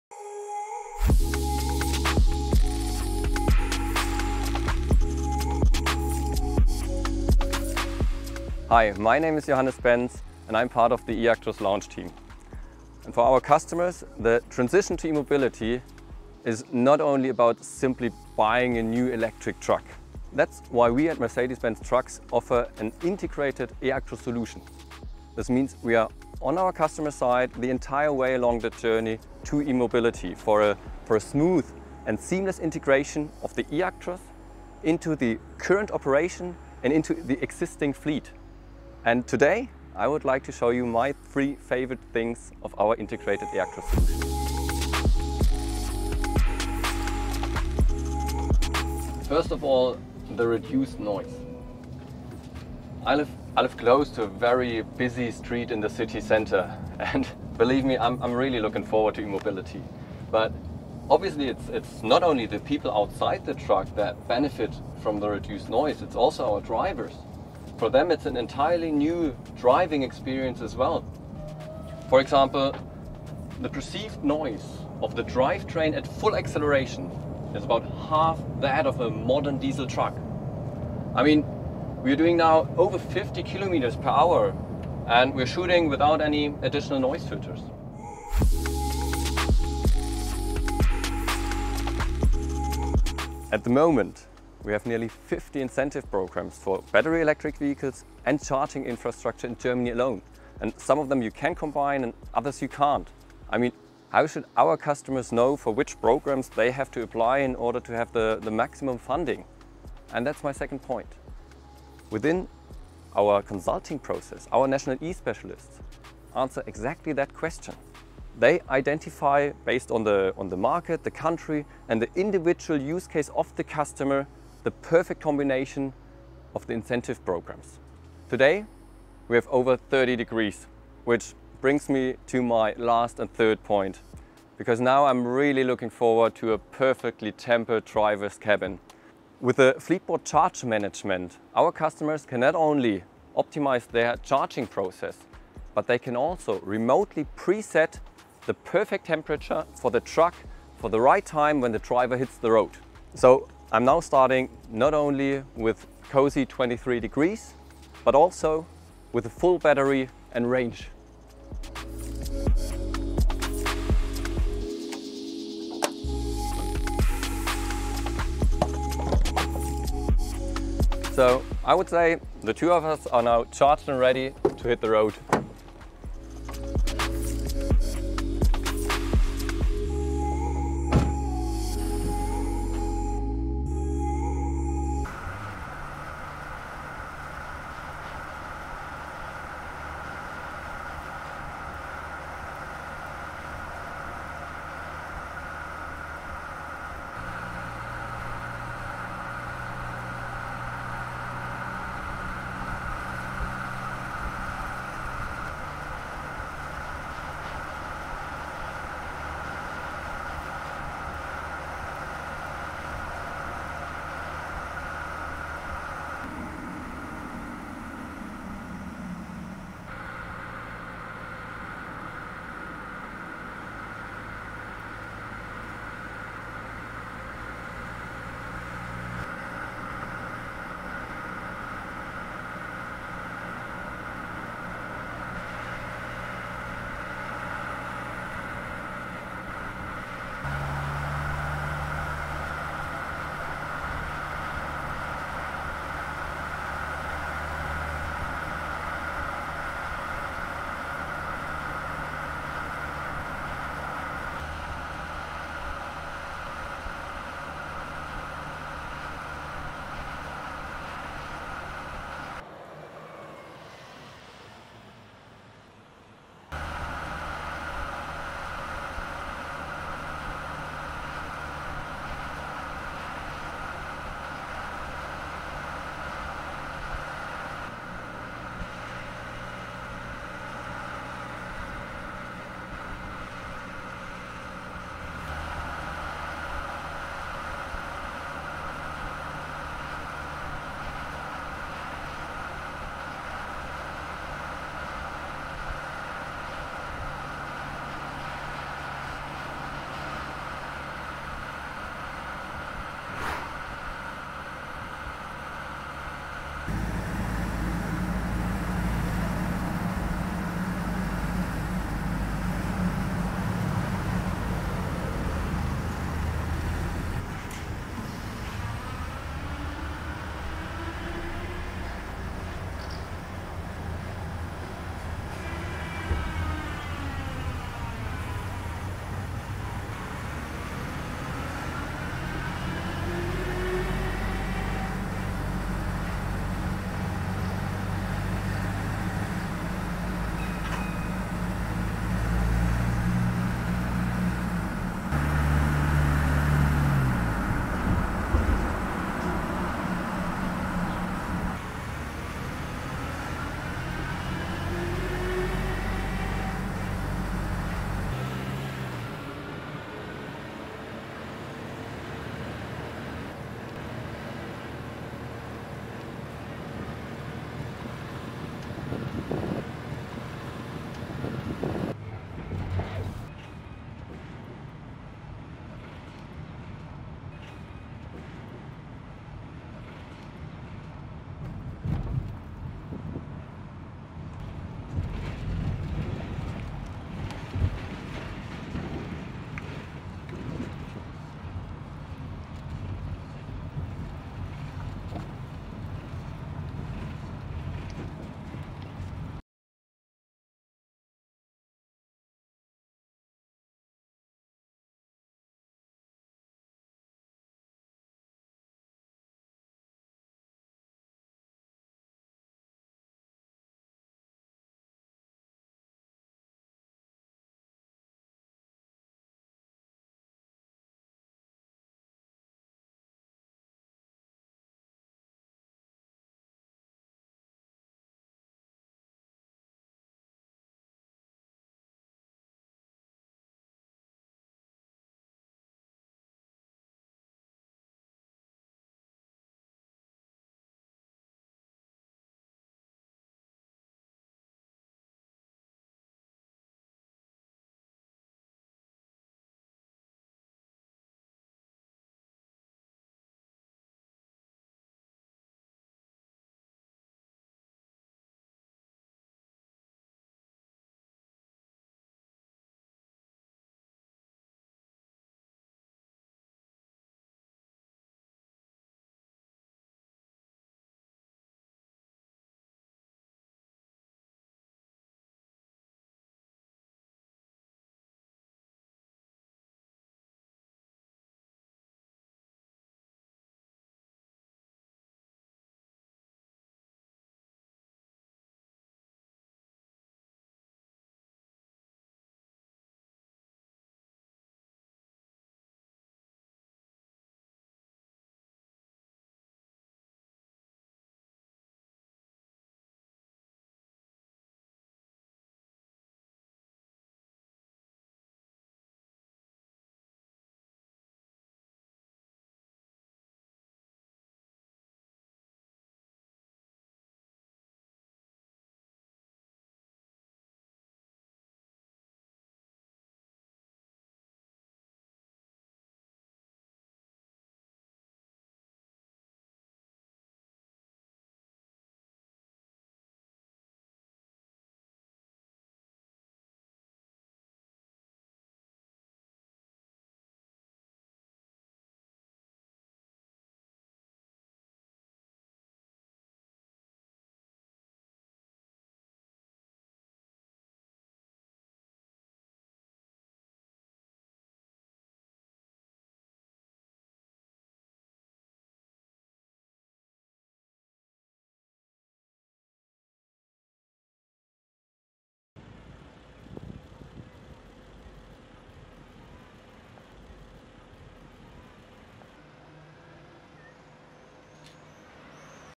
Mercedes eActros Electric Truck - Official Launch
Range of up to 400 kilometres, battery capacity of around 420 kWh[2]
Electric axle with two powerful electric motors delivering a top output of 400 kW
Impressive driving dynamics and high driving comfort thanks to a truly responsive two-speed transmission
Tailor-made turnkey charging solutions thanks to comprehensive advice and service offering.
Intelligent digital solutions and apps for even greater efficiency and customer benefit
Equipped as standard with proven, innovative core products such as MirrorCam and Multimedia Cockpit Interactive
Safety with high added value thanks to standard features such as the Acoustic Vehicle Alerting System, Sideguard Assist S1R and fifth-generation Active Brake Assist emergency braking system